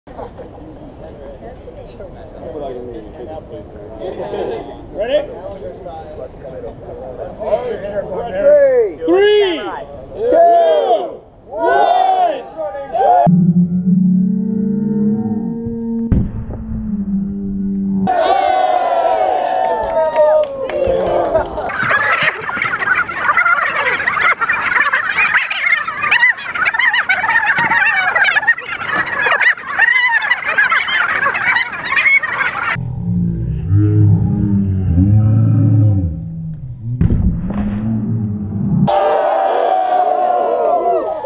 The Melon Smash of (B)East Coast 2014
Mark's two Watermelon smashes from this year's (B)East Coast Jam.